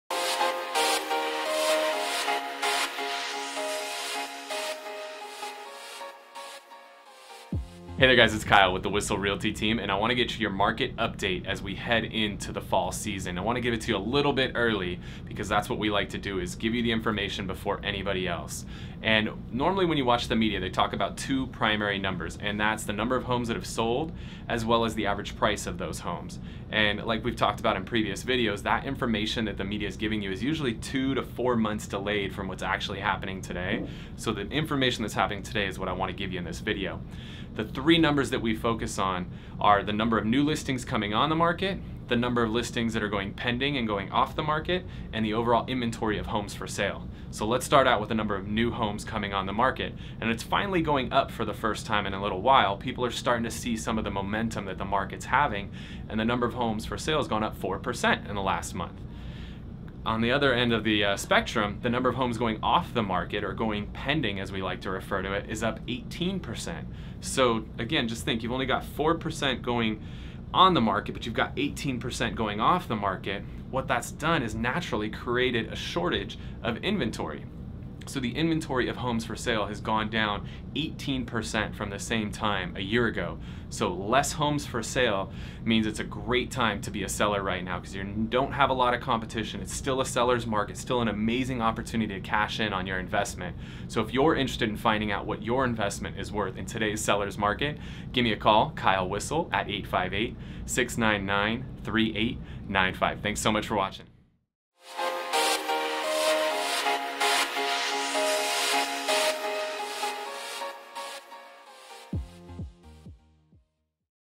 2016 Fall Market Update - San Diego Real Estate Agent
Fall is almost here, so I wanted to give you a quick market update. Keep in mind that when the media talks about real estate, they tend to focus on sales price and the number of home sales. Not only do these two numbers not give you a complete picture of the market, the media often discloses information that is outdated by two to four months. To find out what is currently happening in our market, watch this short video.

Kyle Whissel
Whissel Realty - CA DRE# 01525597
3180 University Ave, Suite 600